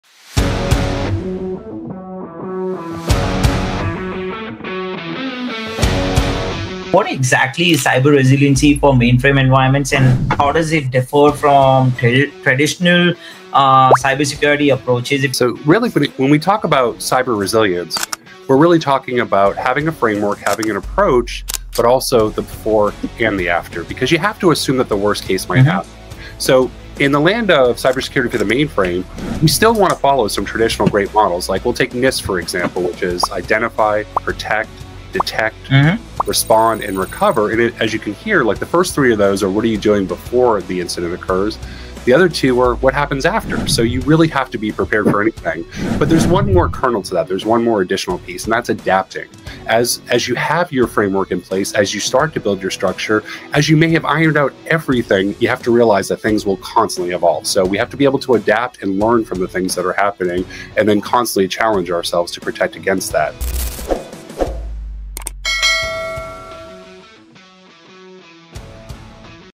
What is cyber resiliency for mainframe environments?
I had a chat with Ryan Howard from BMC Software AMI Security on building cyber resiliency for mainframe environments. Simple, practical, and focused on what actually works in the real world.

What we covered:

• What cyber resiliency really means for mainframes
• The core controls that matter: prevention, detection, isolation, recovery
• Common blockers teams hit when rolling out new controls
• A real incident walkthrough and how recovery stayed on track

Who should watch:

• Security leaders who own mainframe risk
• Infra and ops teams running mission-critical workloads
• Anyone tightening their ransomware playbook